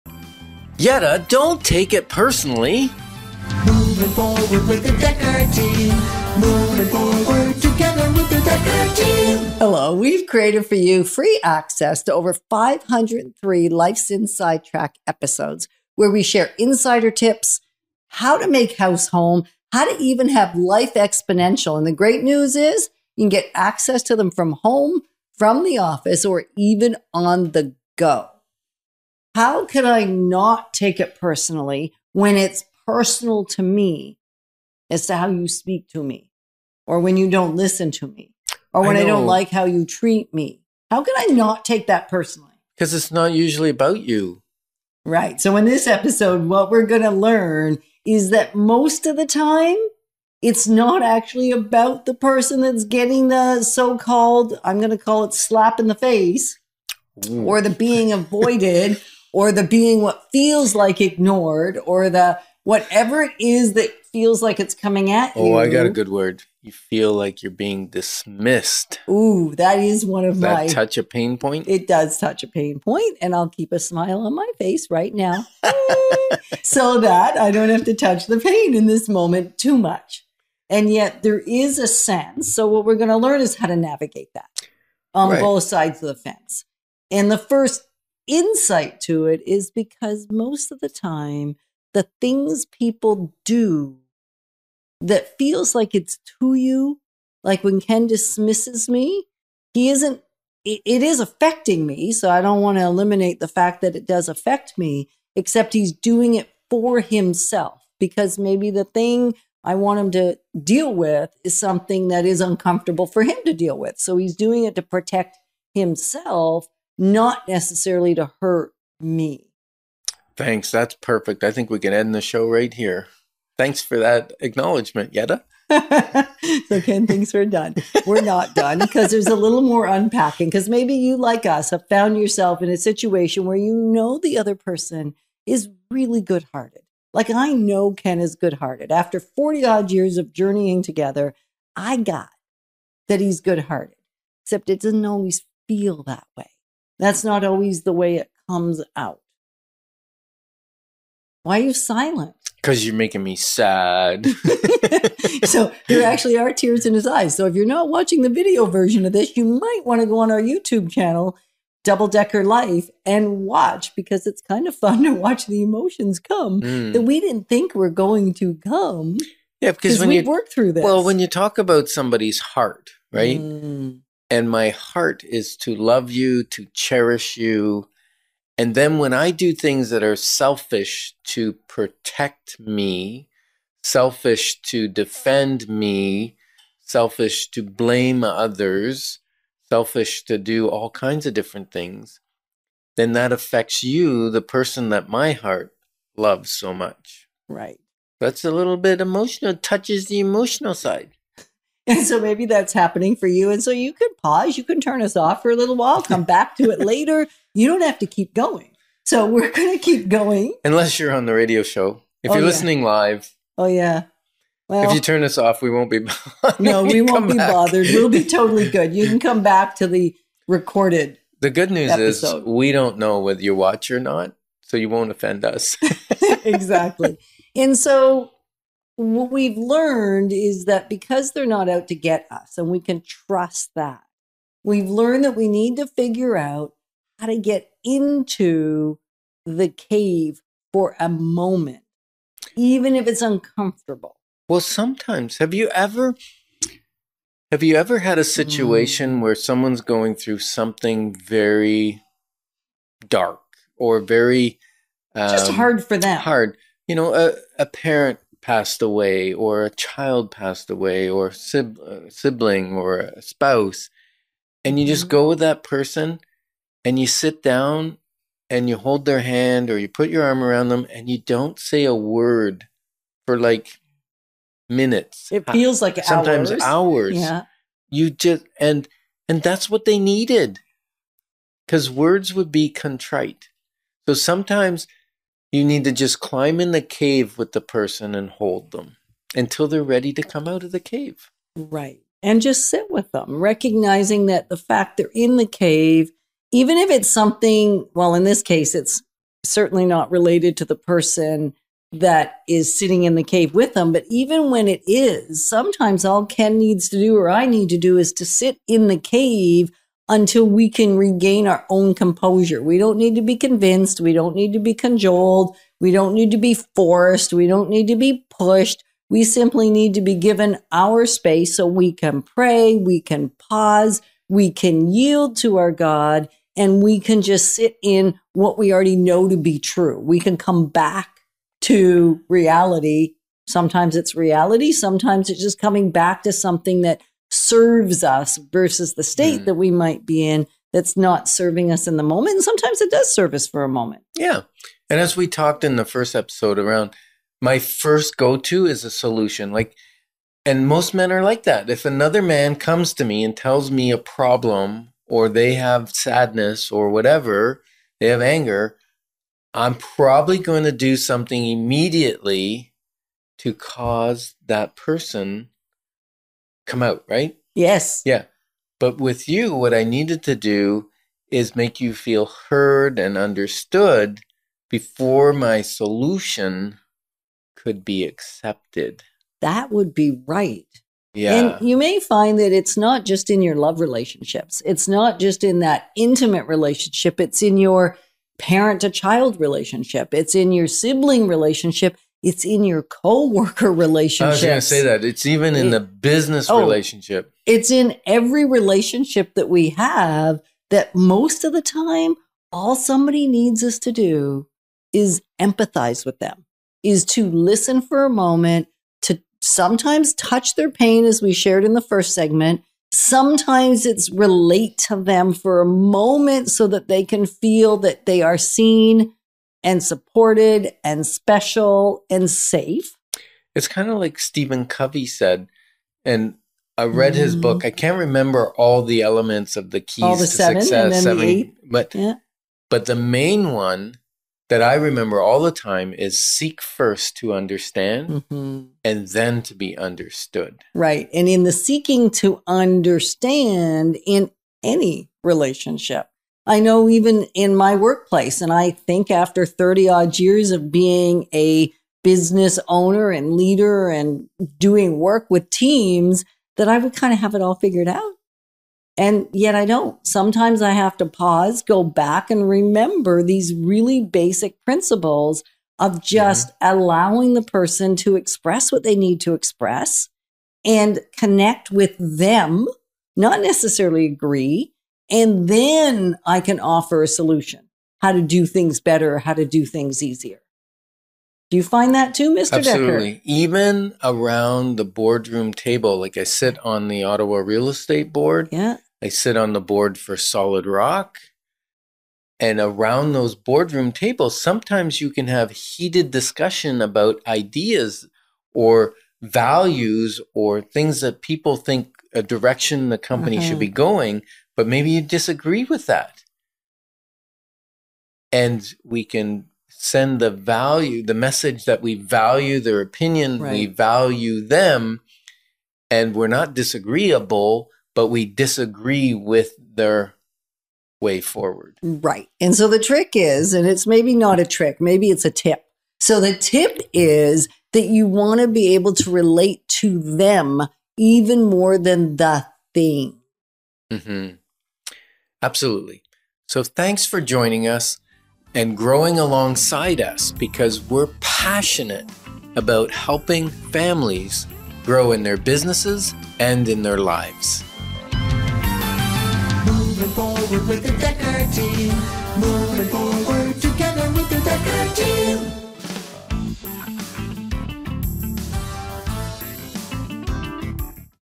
I’M PERSONALLY OFFENDED - I’M ALL EARS or AM I
I’M PERSONALLY OFFENDED  Do you find yourself often offended in relationships? Do you struggle with not taking things personally? In this episode, hear from Yetta and Ken on how they, over the last 30 years, have learned to navigate personal offences even in a relationship they can trust.
 I’M ALL EARS or AM I? Do you find that you struggle with connection in your relationships? Do you feel like you aren’t being heard by those around you? Do you feel a willingness to listen to others? In this episode, we discuss the complexities of communication in relationships and how we can learn to infuse harmony and LIFEexponential with those around us.